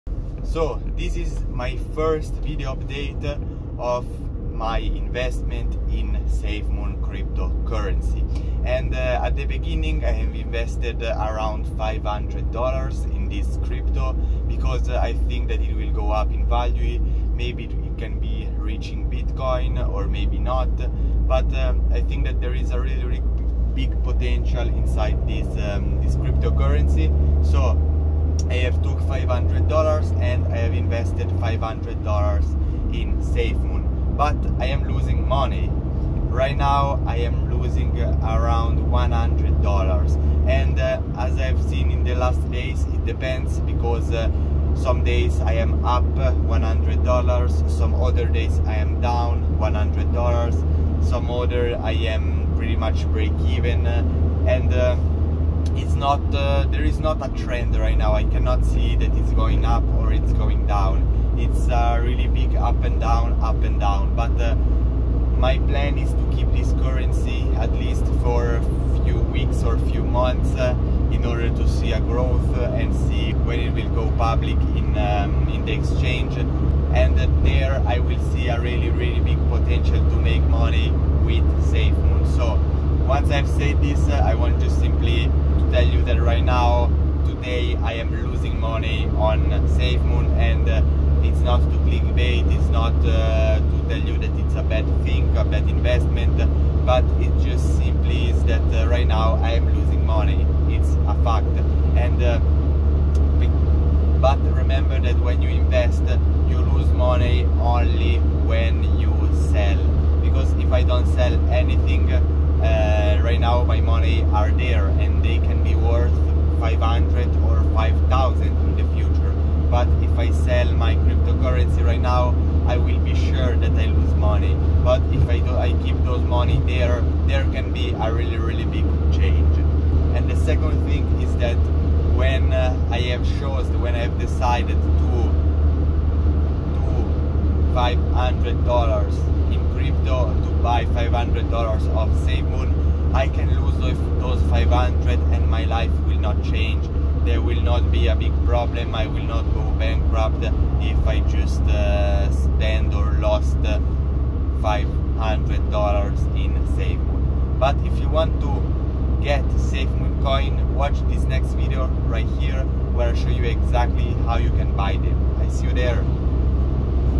SAFEMOON: I’m Losing money! (1° update)
This is a video update of my investment in the crypt SAFEMOON